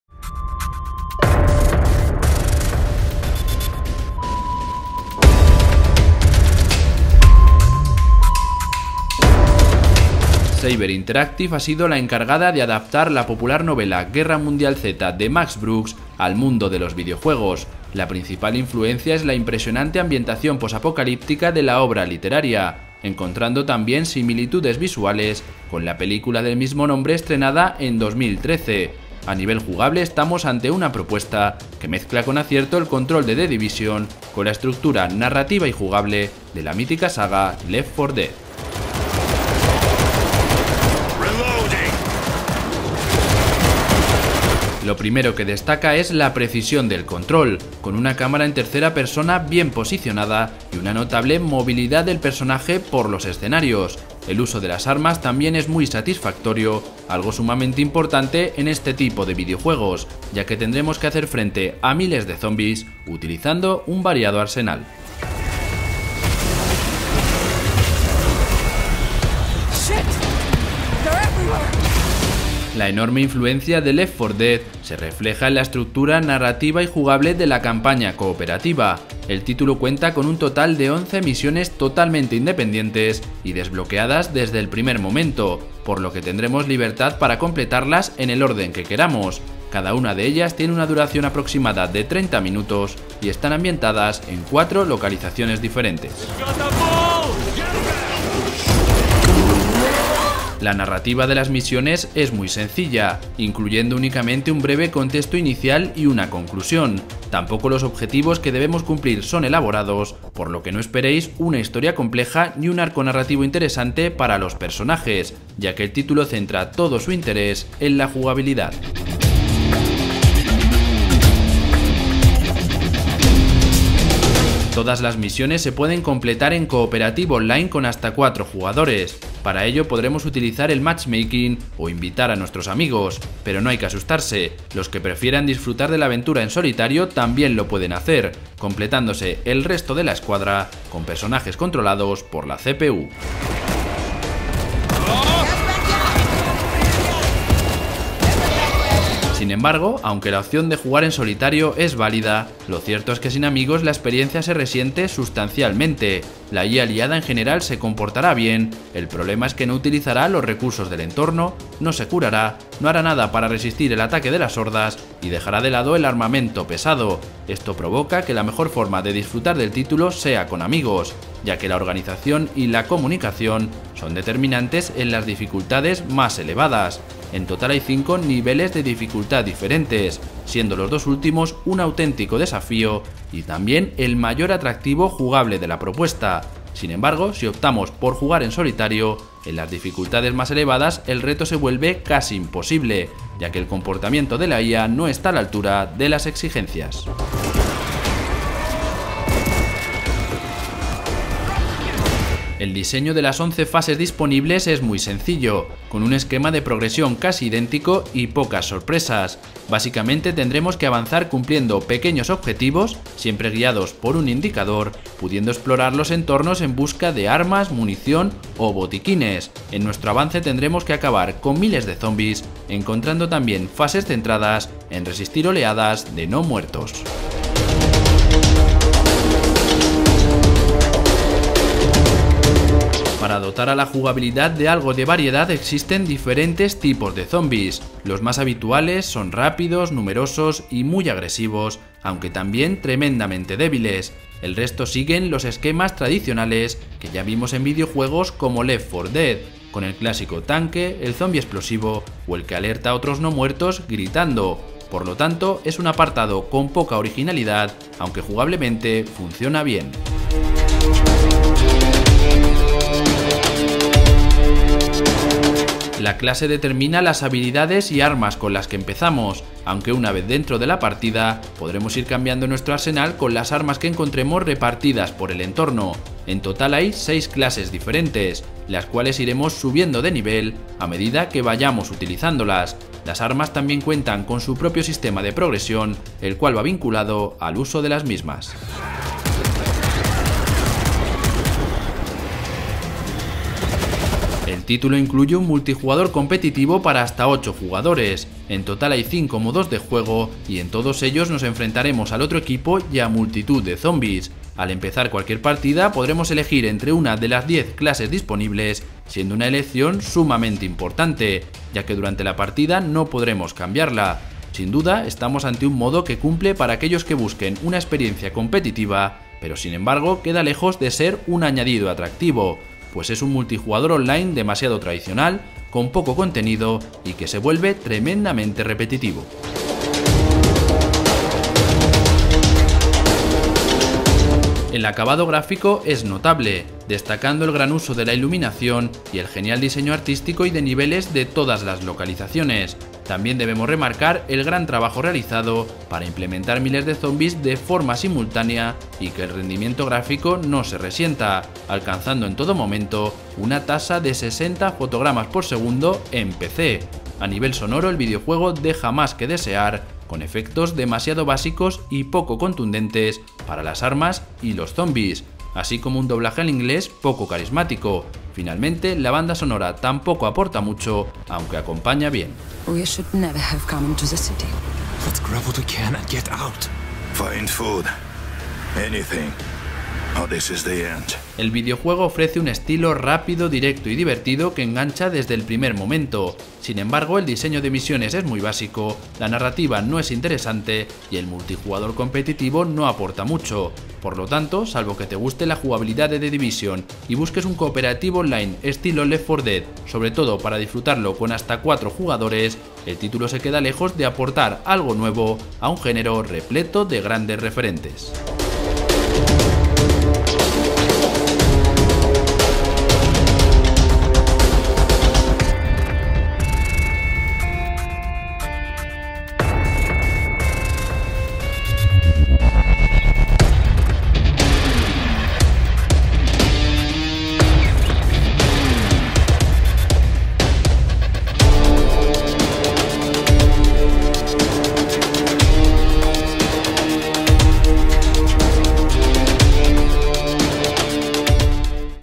ANÁLISIS WORLD WAR Z (PS4, Xbox One, PC) ACCIÓN COOPERATIVA con VIRTUDES y MUCHAS CARENCIAS
Análisis de World War Z, ya a la venta en PC, PS4 y Xbox One. Saber Interactive ha sido la responsable de adaptar la novela World War Z al mundo de los videojuegos. Estamos ante un shooter en tercera persona que mezcla la jugabilidad de The Division, con la estructura narrativa y jugable de Left 4 Dead, destacando la opción cooperativa online para hasta 4 jugadores. Divertido en cooperativo, la falta de contenidos y las misiones repetitivas se acaban volviendo en su contra, te lo contamos en este análisis.